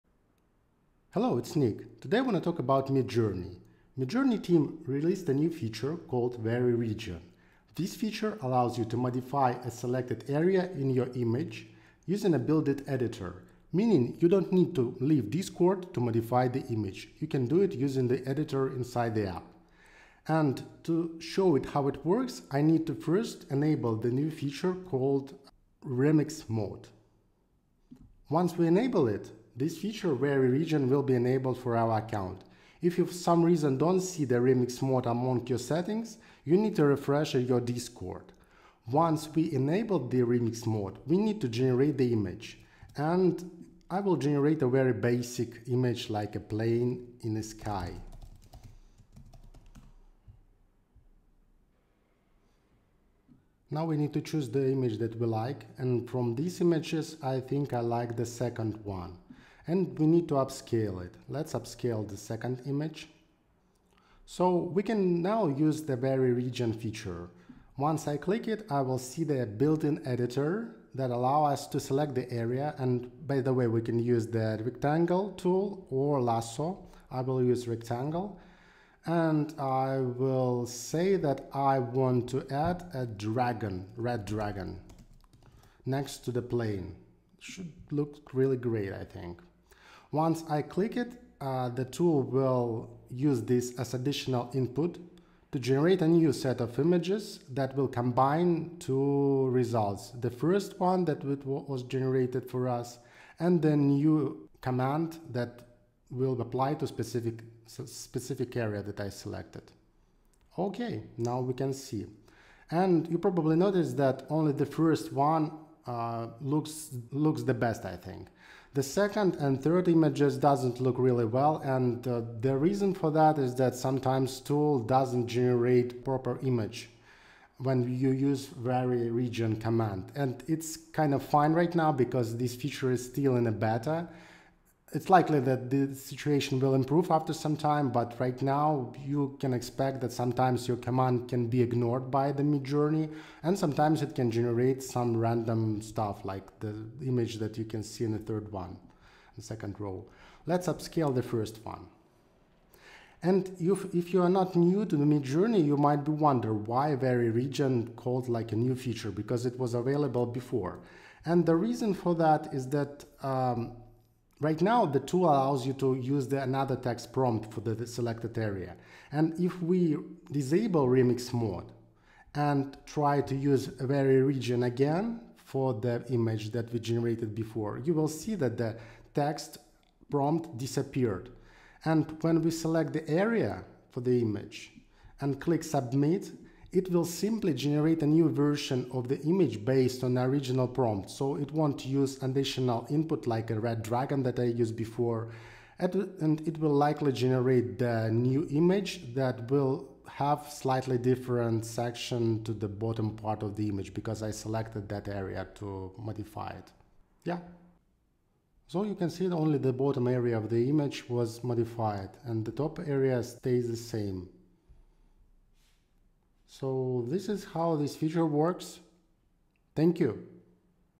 Editing images in Midjourney
Midjourney team released a new feature called Vary (region) that allows you to modify a selected area in your image using a built-in editor. No need to leave Discord; simply select an area and describe in plain words what you want to change in the image. In this video, I will show you how to enable this feature and what results you can achieve using it.